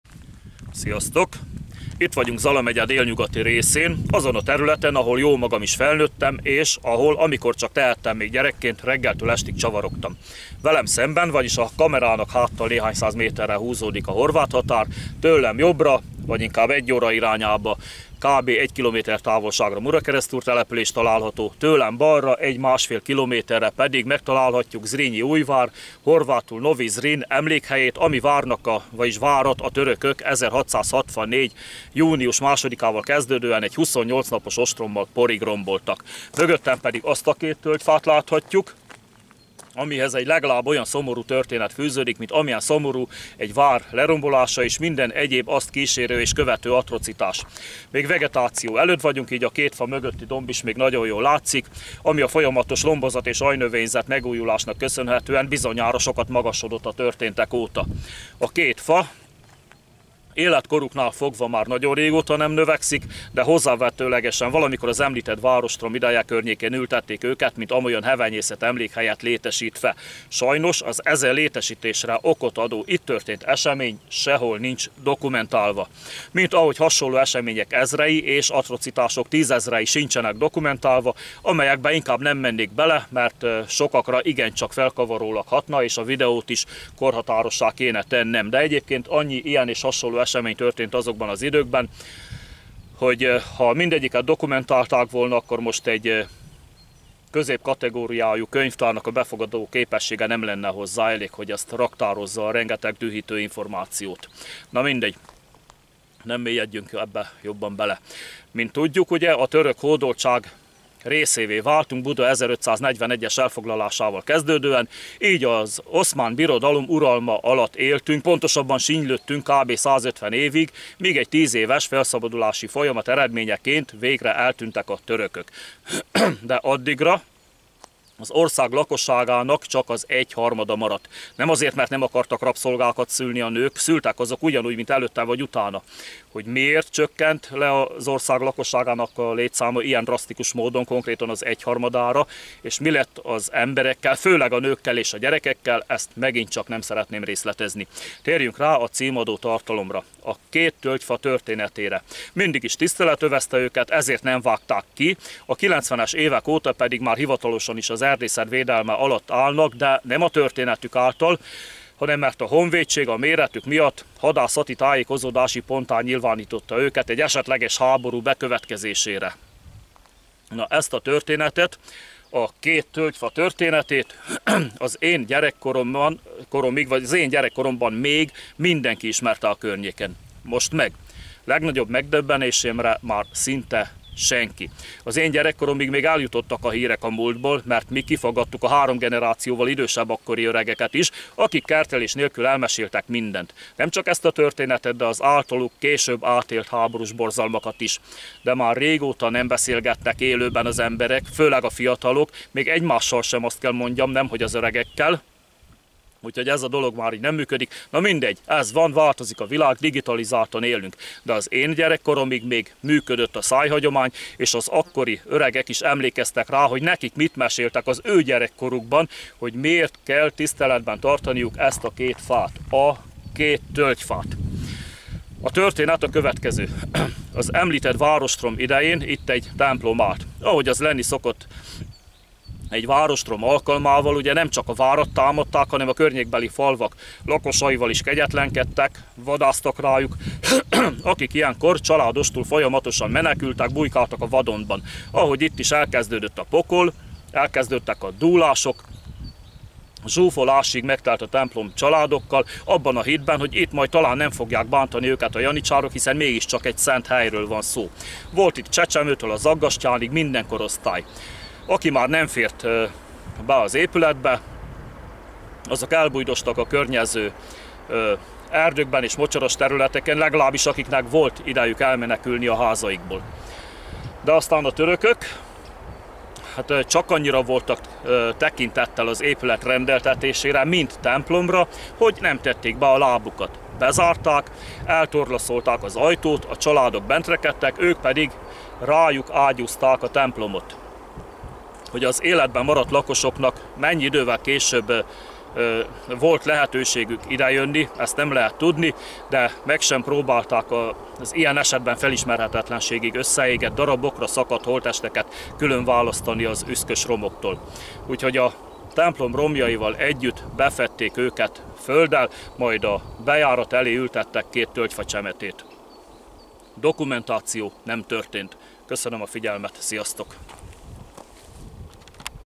147. A murakeresztúri KÉT TÖLGYFA szomorú története a török hódoltság idejéből
Zala-megye délnyugati részén, követlenül a horvát határt követő folyó, a Mura mentén fekvő Murakeresztúr közelében található két öreg tölgy, amelyeket még az oszmán birodalom uralma alatt, a magyarok ellen elkövetett bűnök sokaságának egyike emlékére ültettek a helyi falvak lakosai.